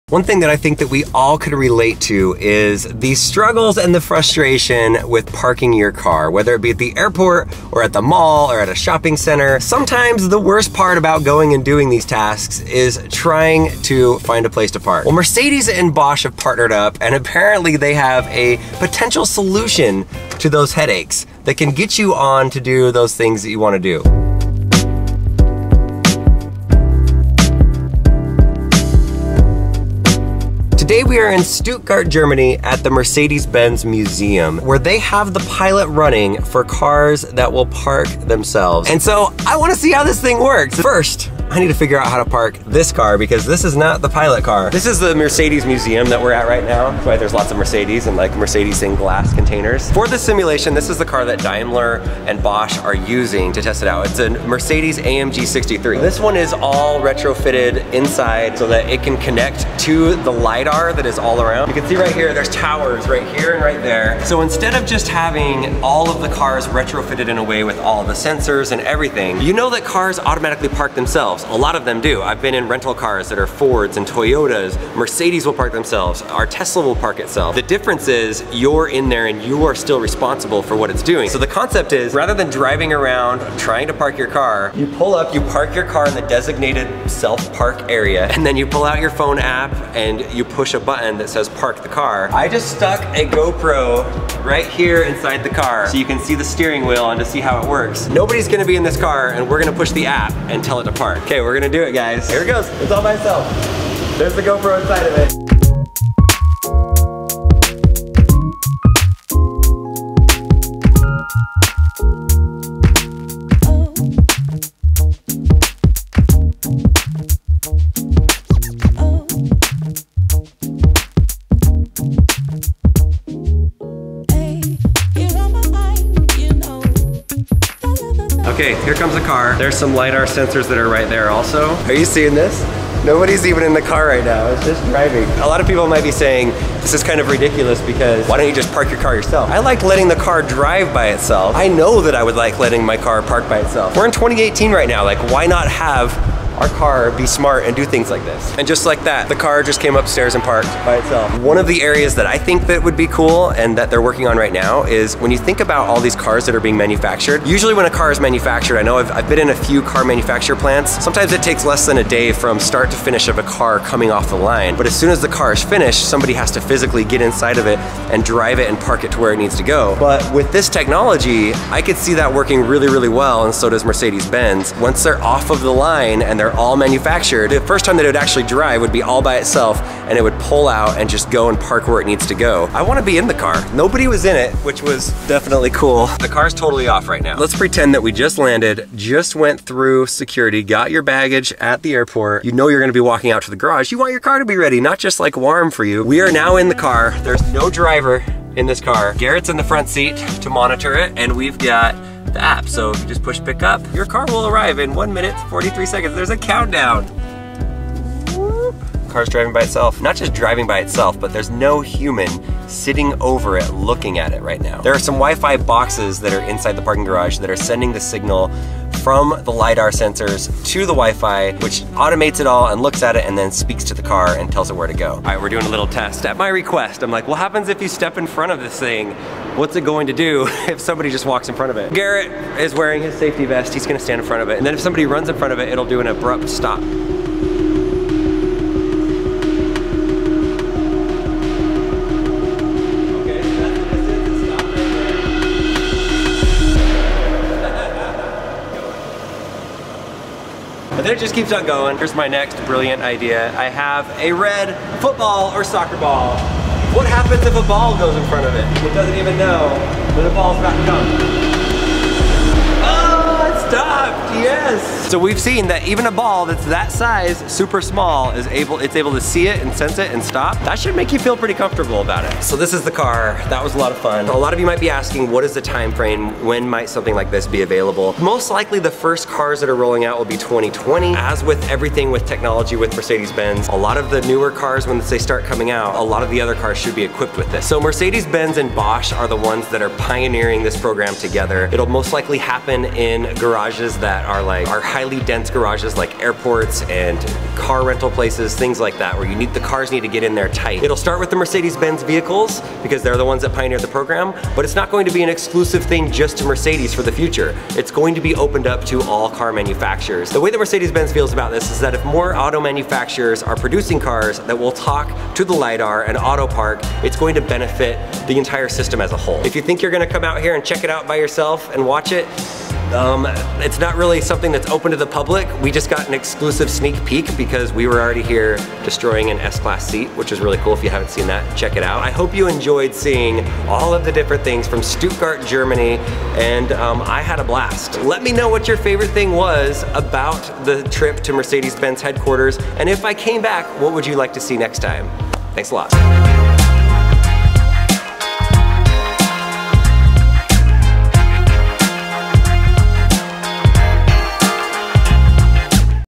The ONLY Parking Garage in the World Like This!
Mercedes and Bosch are Using Lidar to have Cars Park Themselves!

Thanks Mercedes for giving us access to this garage.